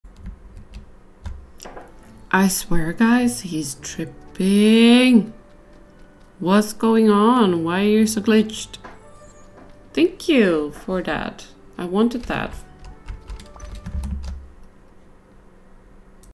Scrooge is glitching
So I just got a Glitching scrooge in the middle of my iron farming